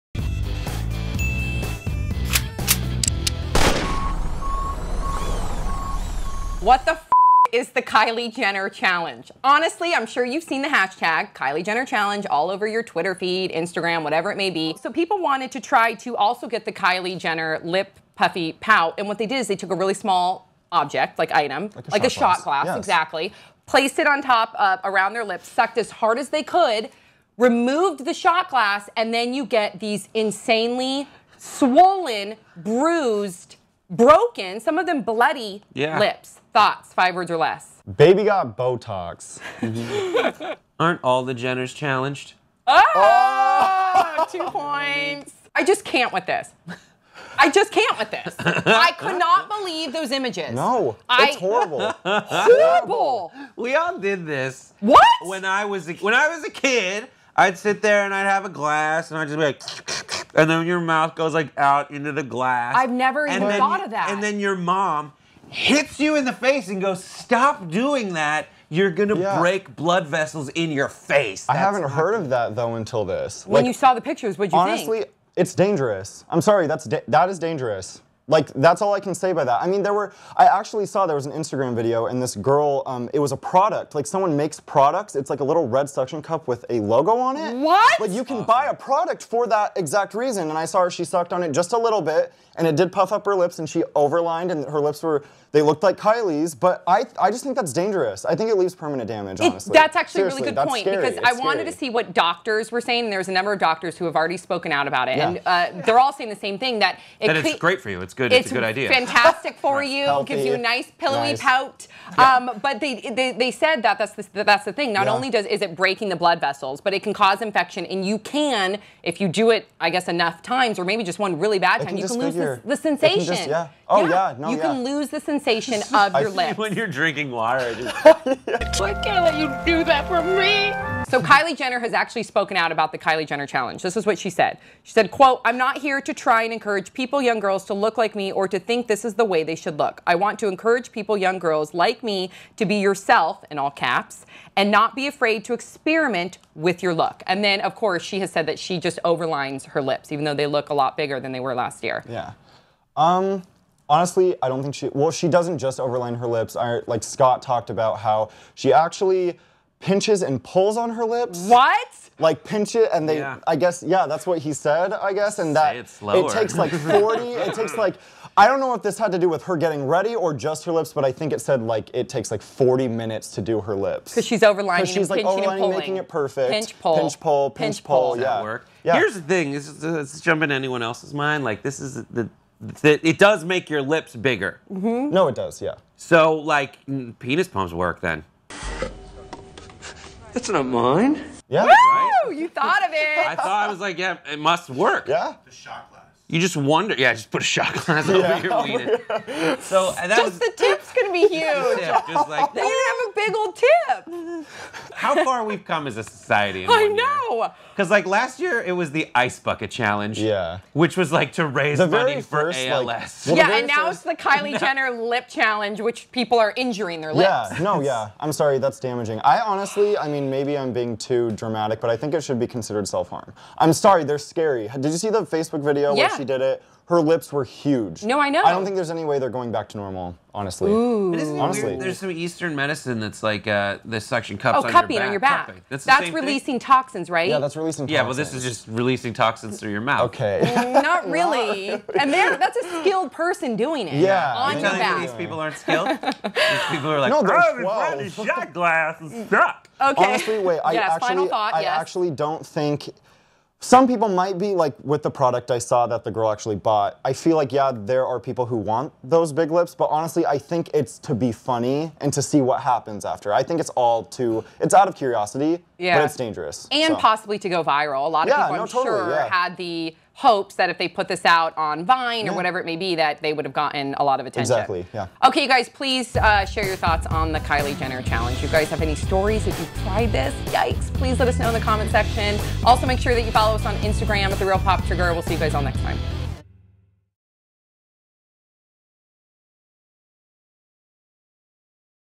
The Kylie Jenner Challenge
What ever happened to the Ice Bucket Challenge?

Samantha Schacher, host of Pop Trigger, Brett Erlich, and Julian Astudillo break it down.

"There was the Cinnamon challenge, the Ice Bucket challenge and, now, the Kylie Jenner Lip Challenge.

Teenagers insert their lips into a shot glass, small jar or bottle, then suck out the air, creating a vacuum. The hope is they’ll end up with pouty lips like the reality TV star. But some attempts have gone awry: girls end up with bruises, soreness and even torn skin.

Canadian dermatologist, Dr. Benjamin Barankin, says there are many ways young girls are damaging their lips from taking on the controversial challenge."